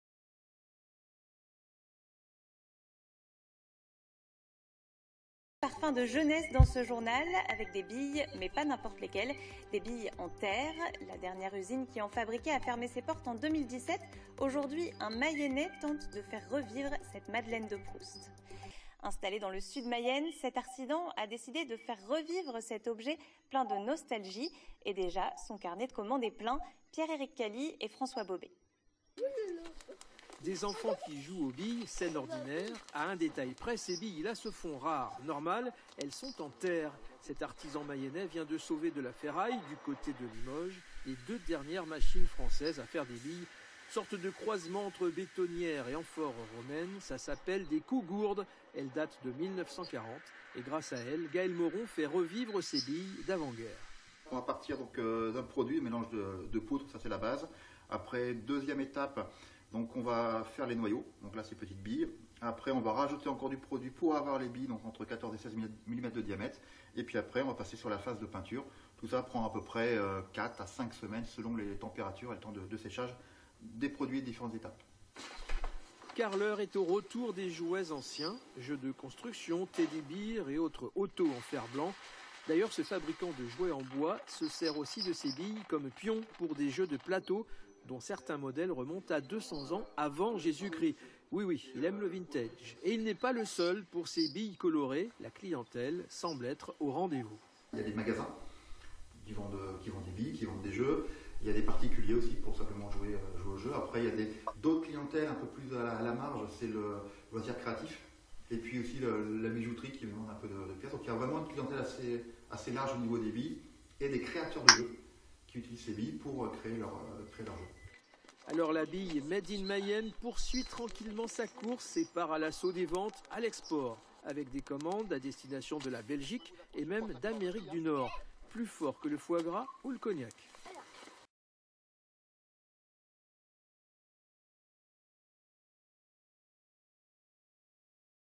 Billes en May sur FR3 Pays de la Loire 2
Le reportage TV dans l'atelier Billes en May à Congrier. Les machines, la fabrication, la peinture (d'un échantillon), les marchés.